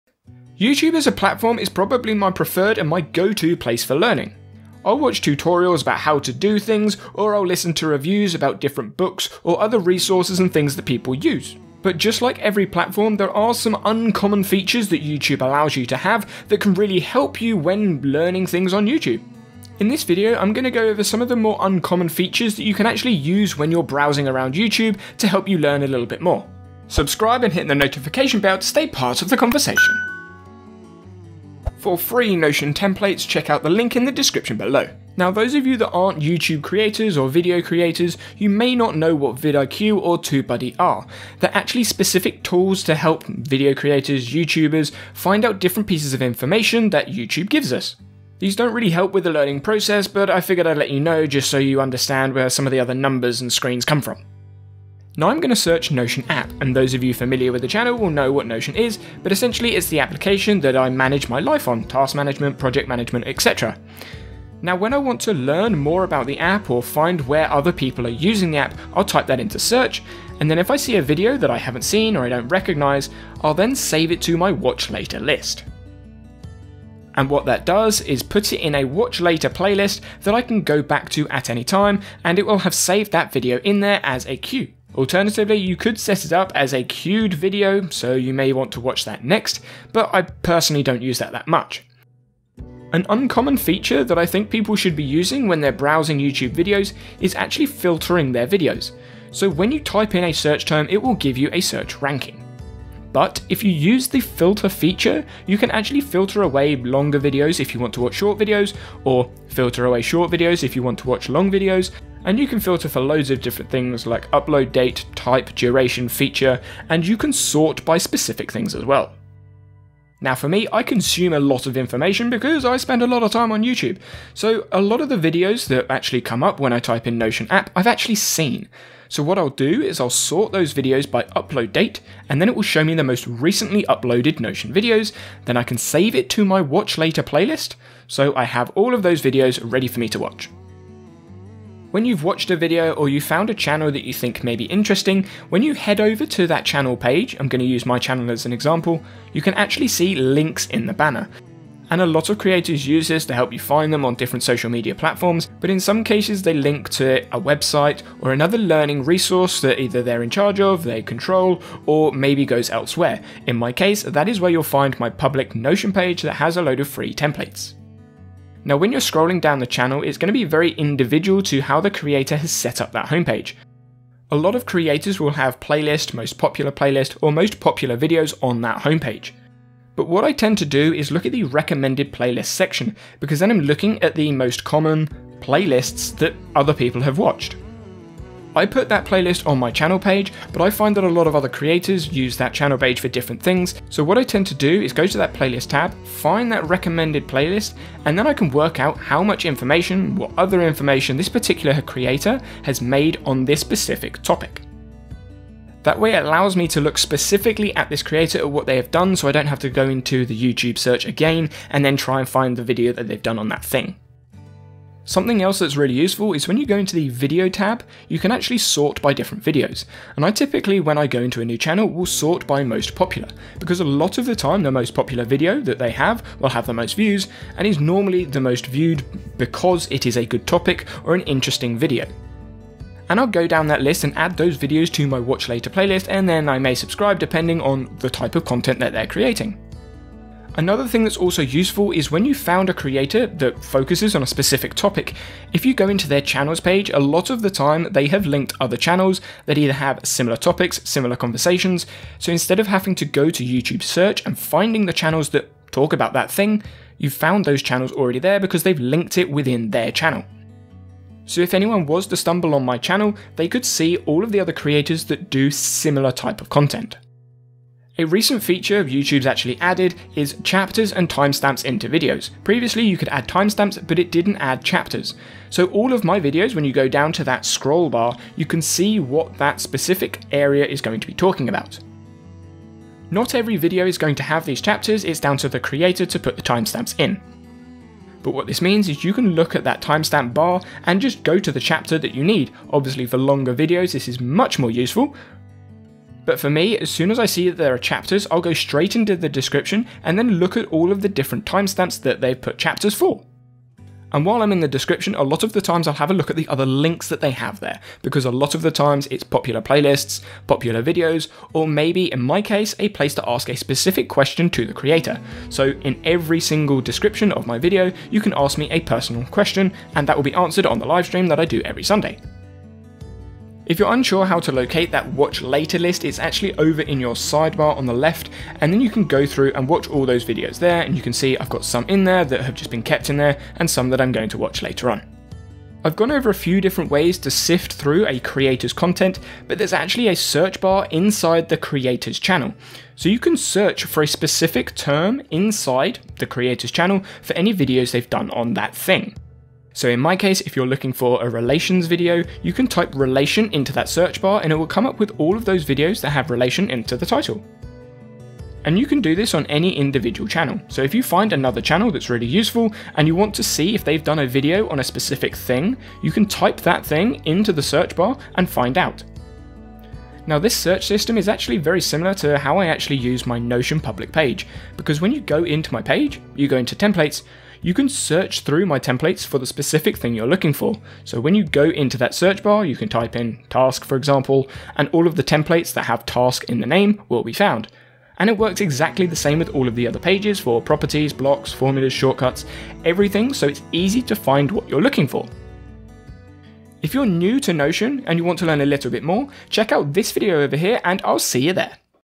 HOW TO USE YOUTUBE For Learning NOTION
Understanding how to use YouTube for learning changed my education. Knowing how to use youtube for beginners sounds obvious but there's are some uncommon features that can really help you grow. Find out more about the Notion app

⏰ TIMESTAMPS ⏰
0:00 Intro
0:55 Watch later list
1:36 Filtered YouTube search
2:30 Channel banner links
3:00 Recommended playlist
3:30 Sort channel videos
4:30 Video chapters
5:40 Channel search bar